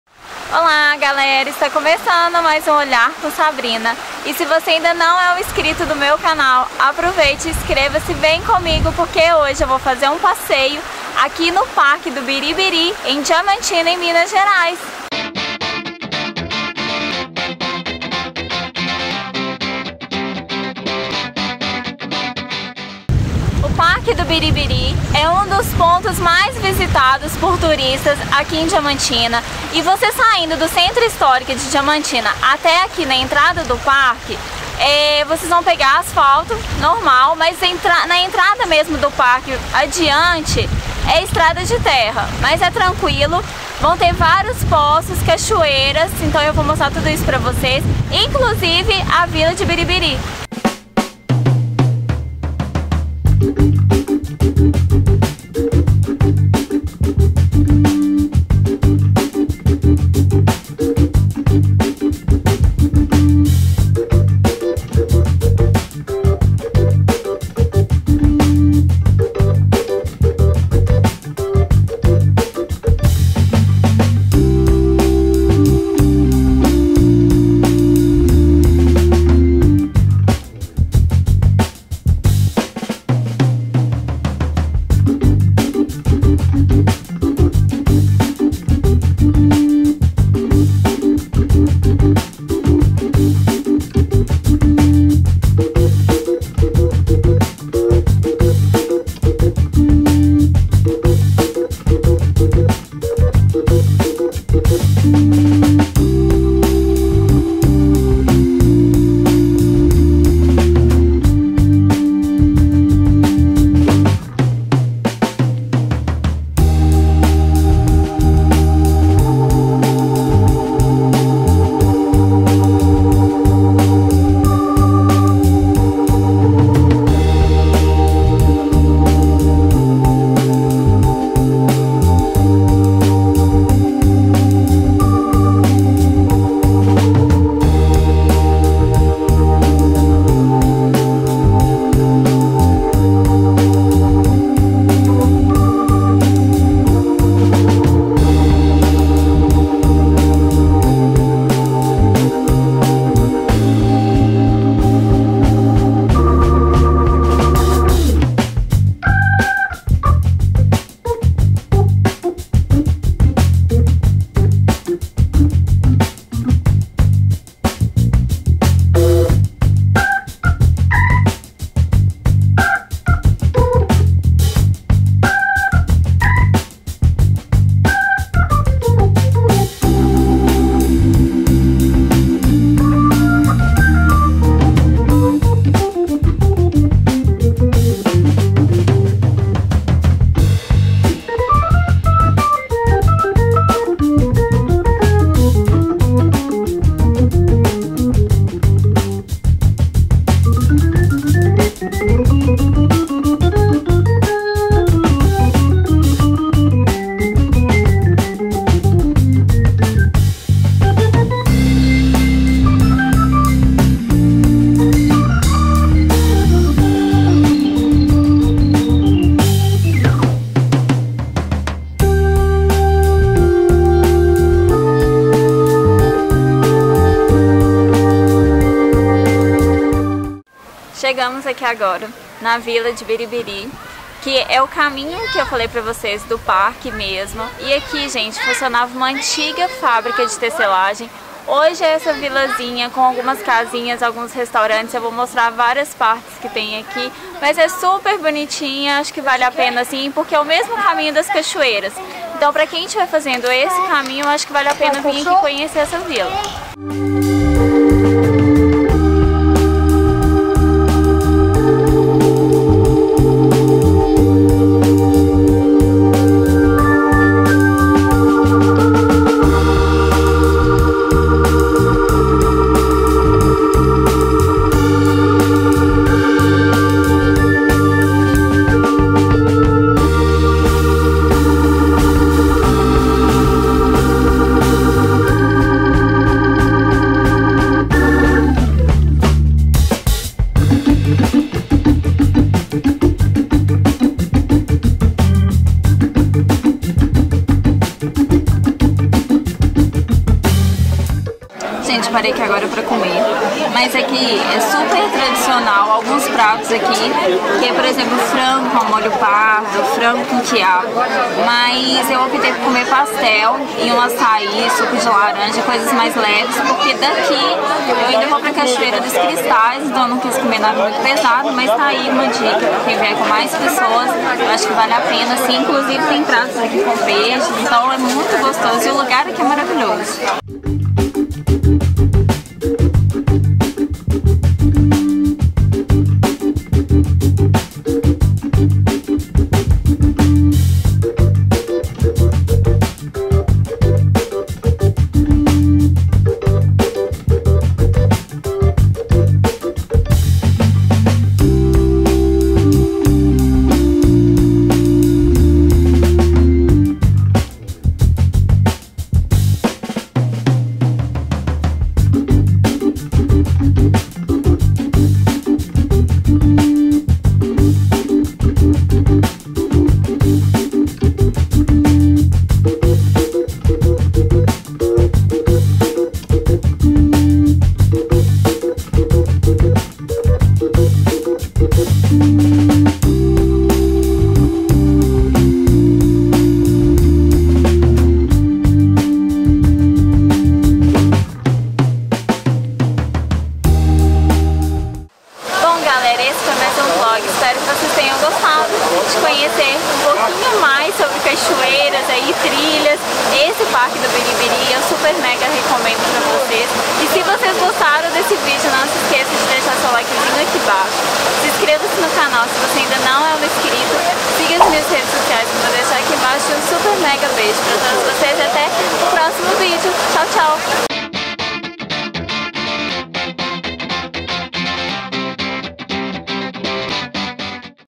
Parque Estadual Do Biribiri - Diamantina MG
O vlog de hoje tem mais trilhas e mais cachoeiras para você!!
Um passeio delicioso pelo "Parque Estadual Do Biribiri" em Diamantina MG. Espero que goste!!!